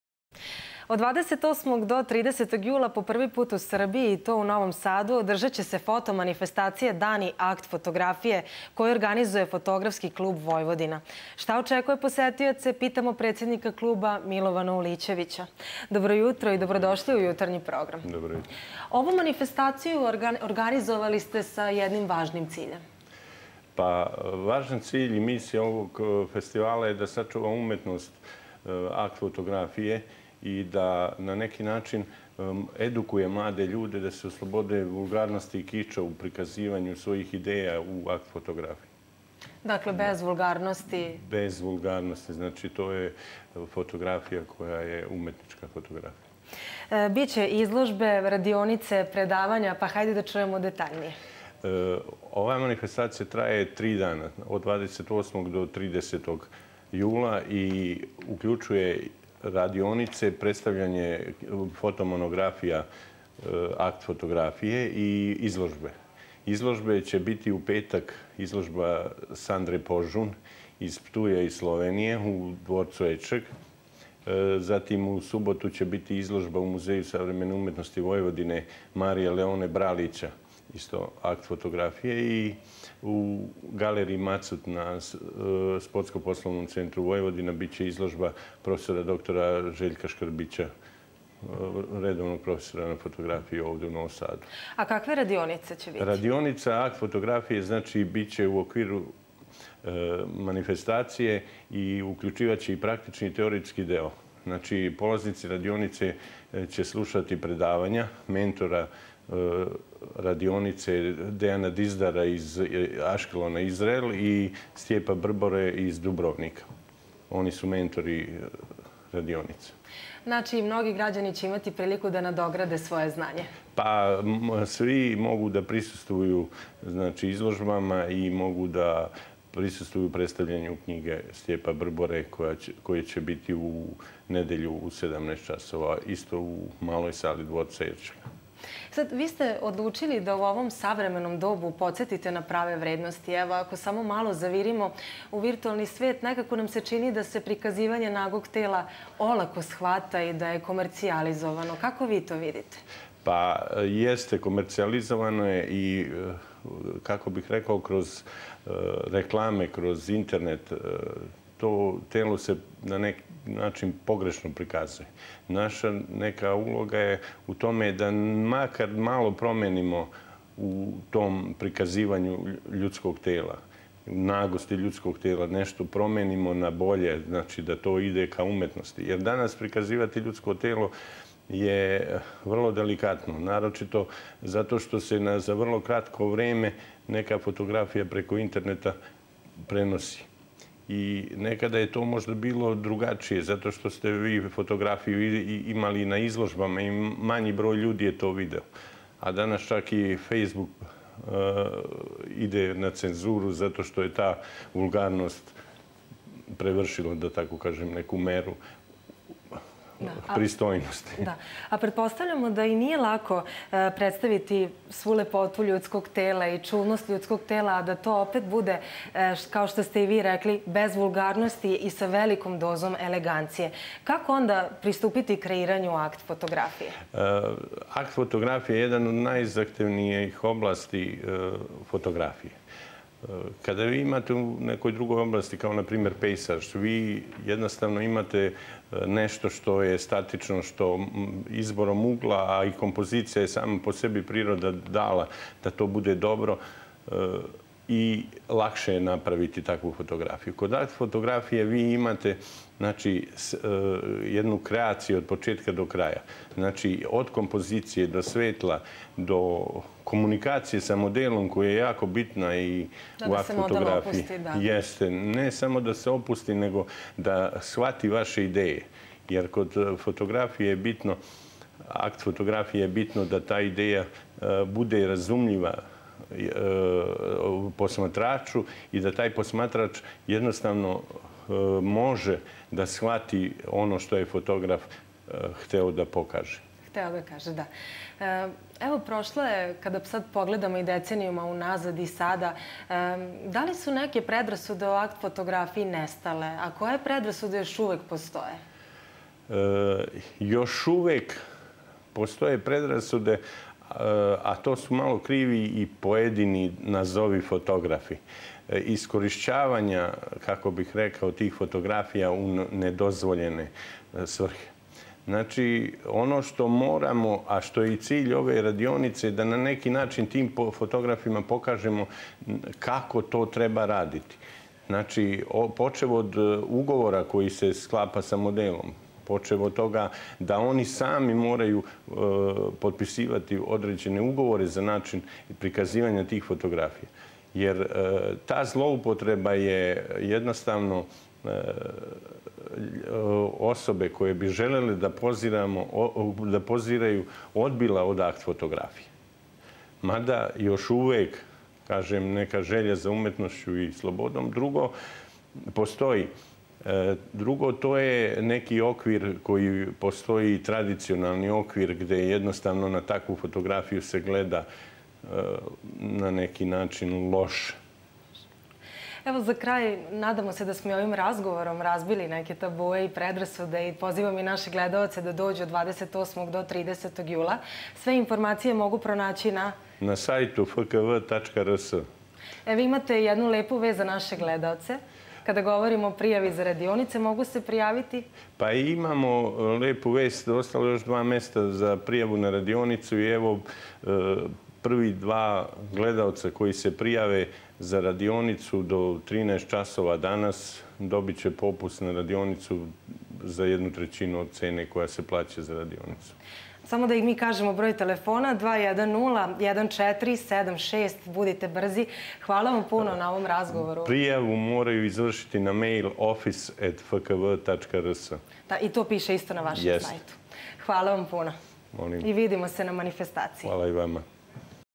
Dani akt fotografije u Novom Sadu
Od 28. do 30. jula po prvi put u Srbiji i to u Novom Sadu, održaće se foto manifestacija „Dani akt  fotografije“ koju organizuje Fotografski klub „Vojvodina“. O tom šta očekuje posetioce, za RTV je govorio predsednik kluba Milovan Ulićević.